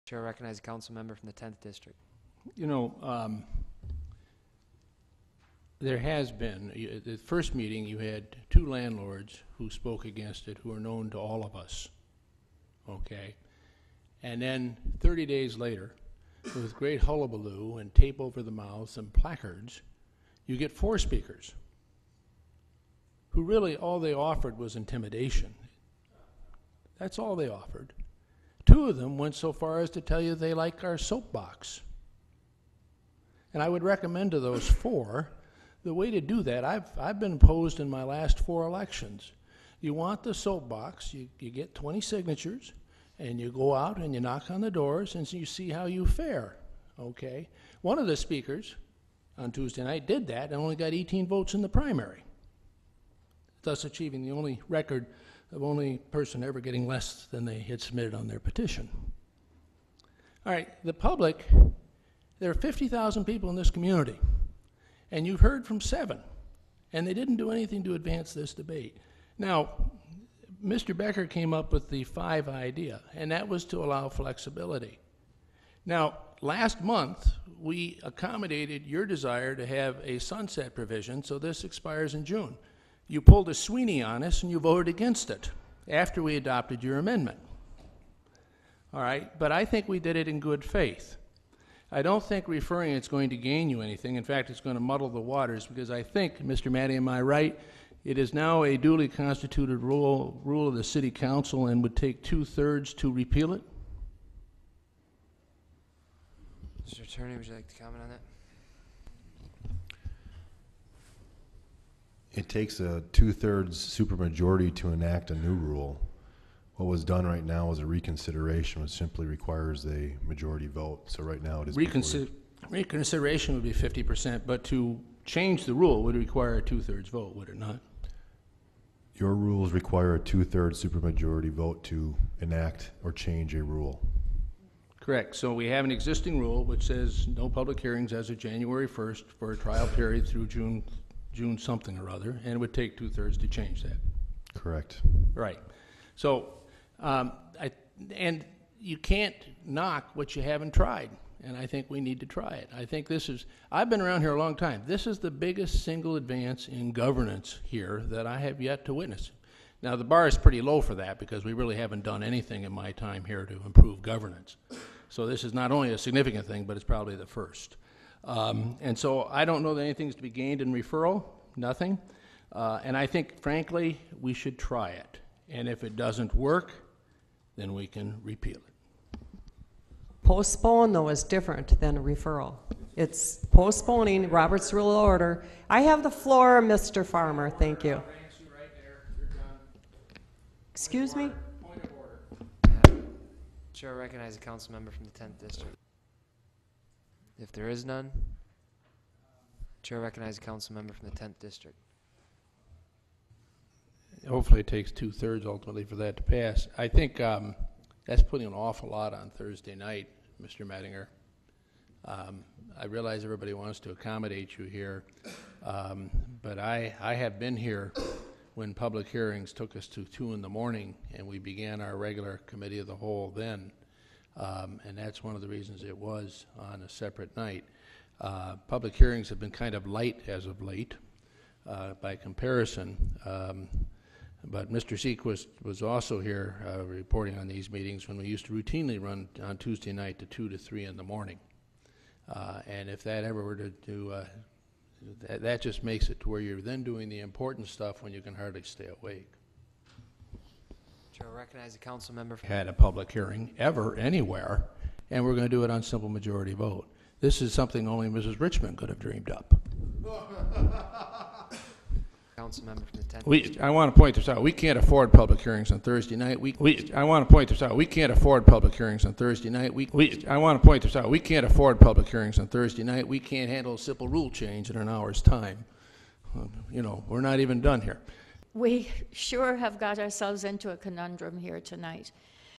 La Crosse Mayorial Candidate Dougie Farmer Argues For No Public Hearings At City Council Meetings
Term